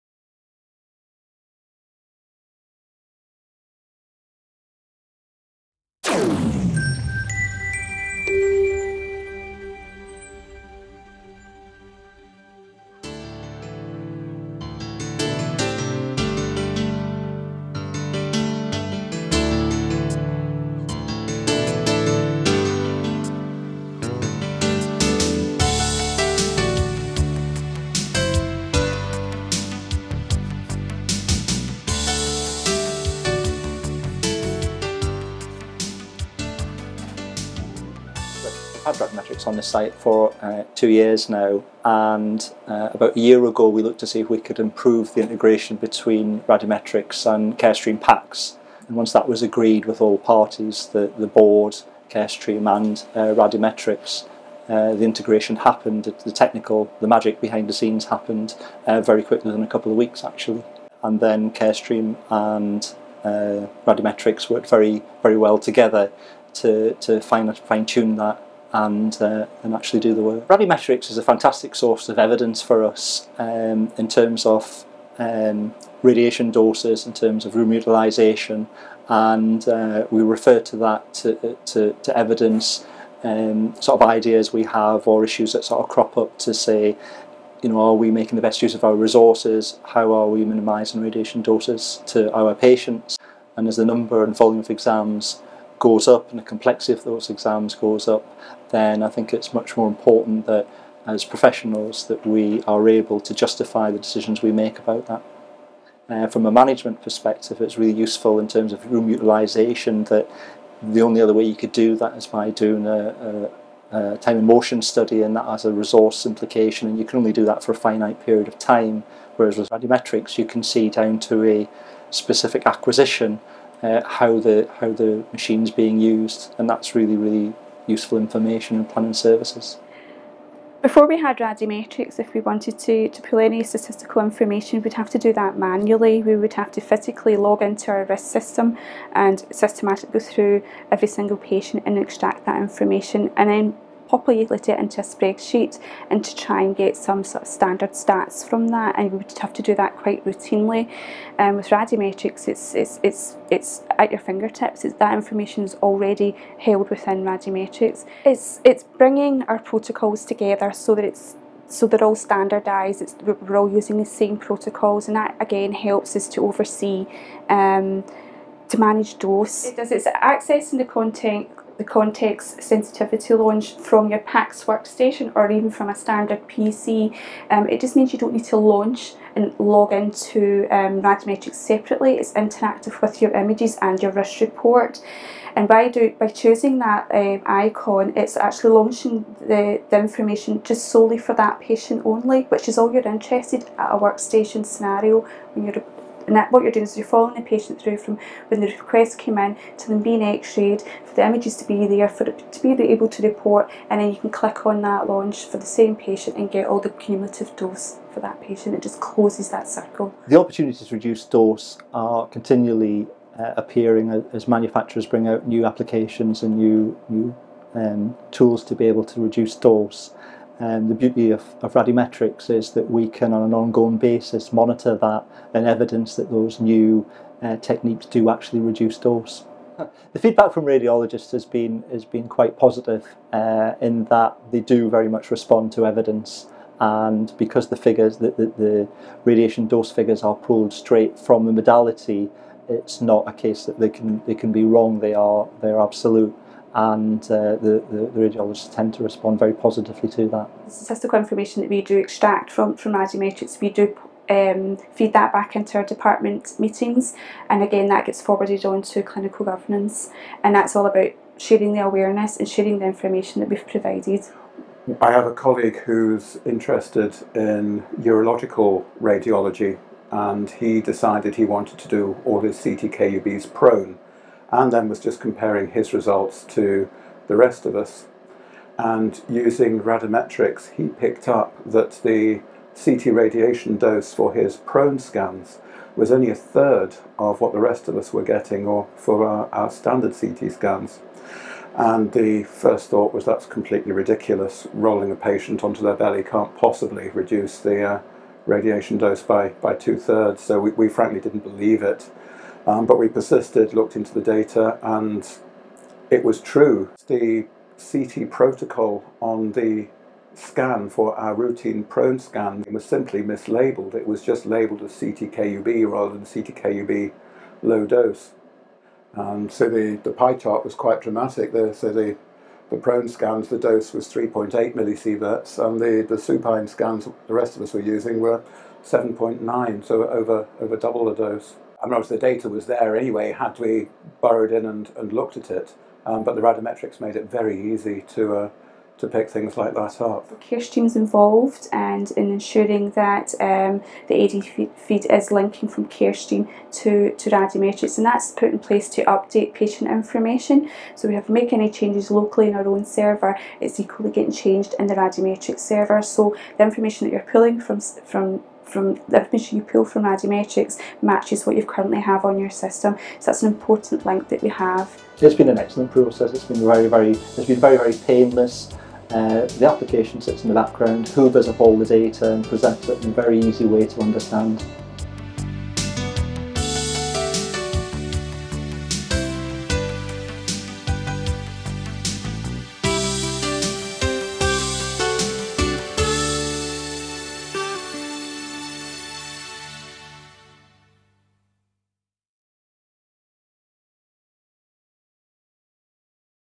Carestream and Radimetrics Working Together with NHS Ayrshire and Arran
Hear how NHS Ayrshire and Arran relied on Radimetrics, integrated into their CARESTREAM Vue PACS, allowed them to track radiation dose thoroughly, and also allowed them to gather this information in a quick, efficient manner. For more information on Vue PACS, go